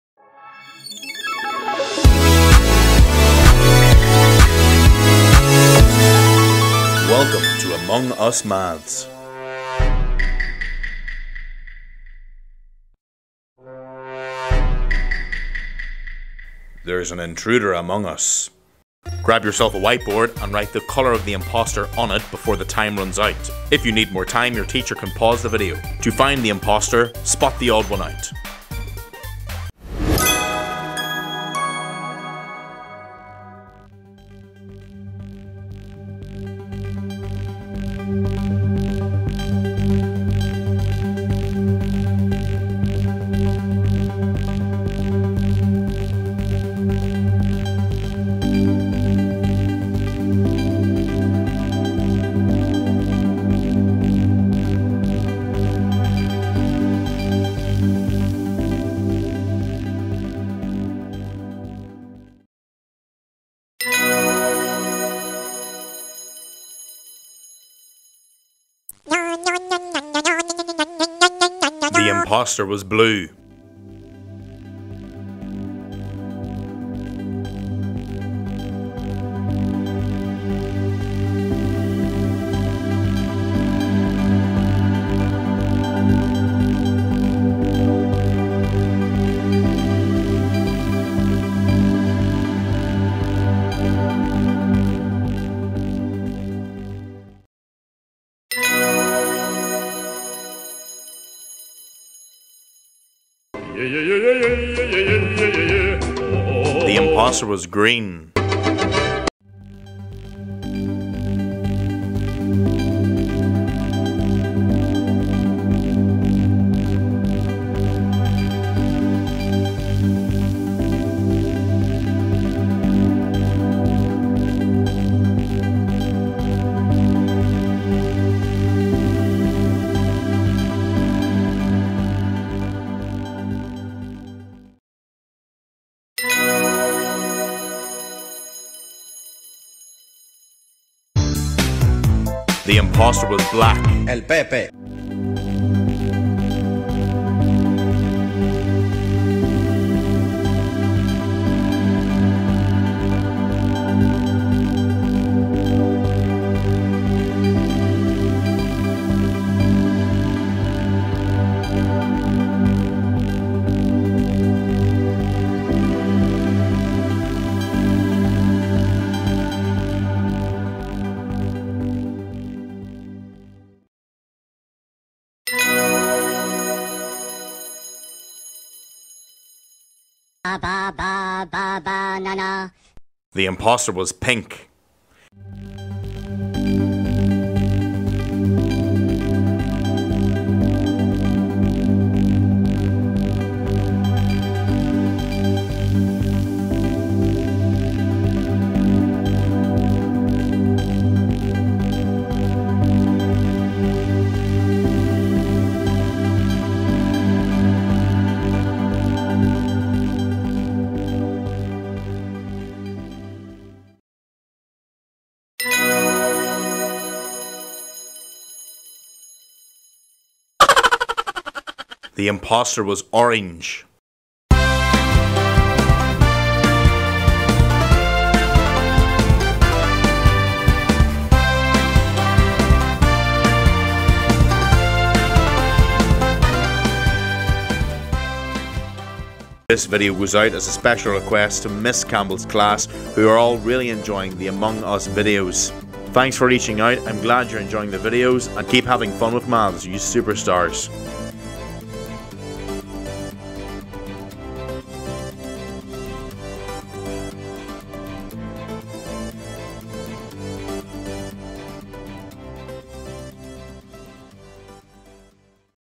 Among Us Maths (Mixed Operations)
Get ready to make your Maths lessons more exciting with Among Us Maths!! In today's video, we are on a mission to spot the intruder among us.

Solve the Maths problems before the time runs out to spot the odd one out.

Use this video as a starter activity in Maths lessons. It's guaranteed to make your lessons more active and fun while helping to develop problem-solving skills and kids' number facts recall.

How to Play:

1. Grab a Whiteboard and marker
2. Solve the 4 Maths problems before the time runs out.
3. Find the odd one out and look at its corresponding colour.
4. Write the colour on your whiteboard.
5. Reveal and check your answers.

Have Fun!

Made from story blocks